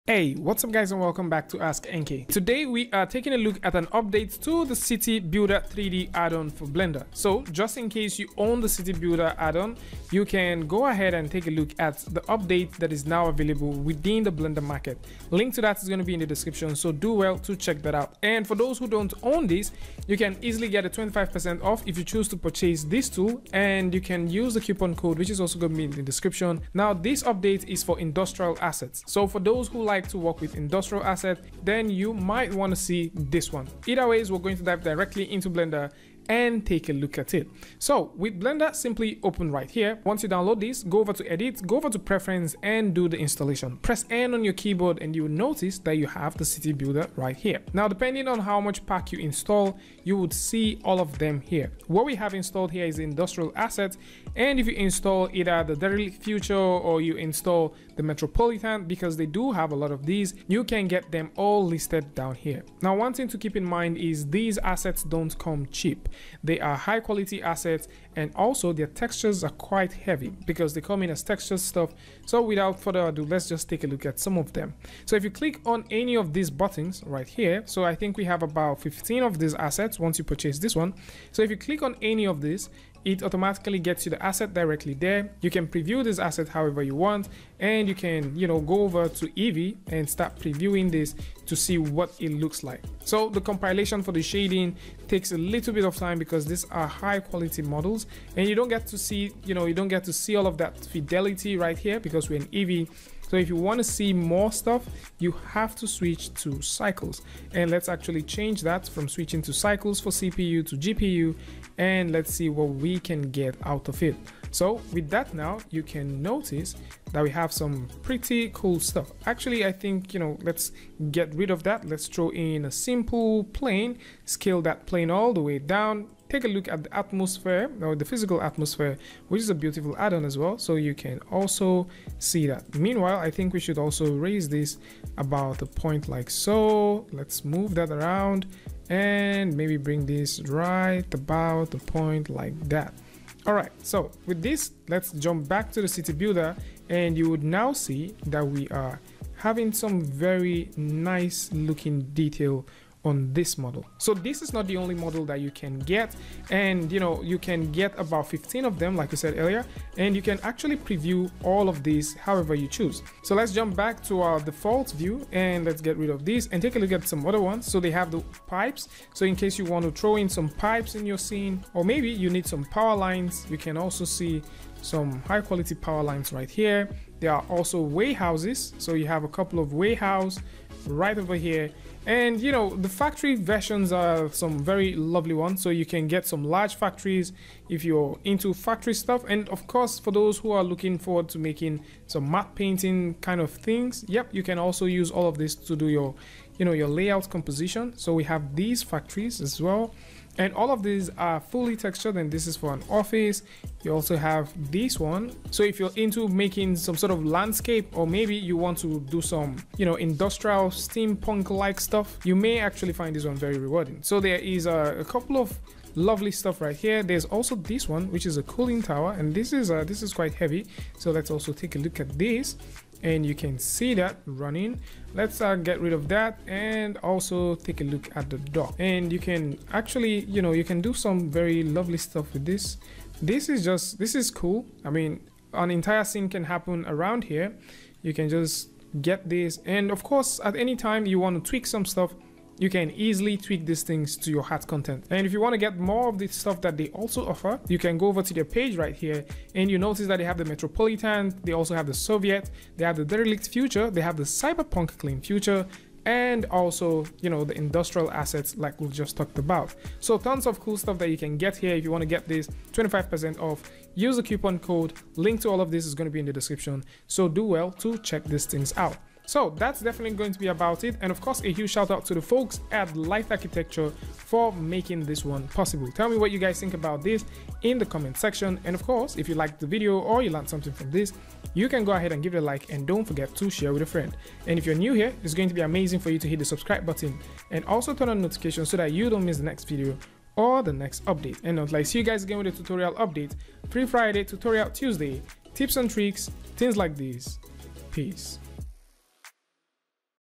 Blender City-Builder - Industrial Asset Addon!
Create custom cities in Blender 3d with over 70 professional 3d assets packed into a single user-friendly add-on! Designed by 3d artists in Hollywood, the CityBuilder add-on and 3d asset database allows you to add over 70 various 3d modeled and textured building assets to your 3d scene with the click of a button! Enhance your 3d art and visual effects work using these professional assets packed into a great add-on with ease! *

** Instagram: @anselemnkoro  -  Twitter: @anselemnkoro **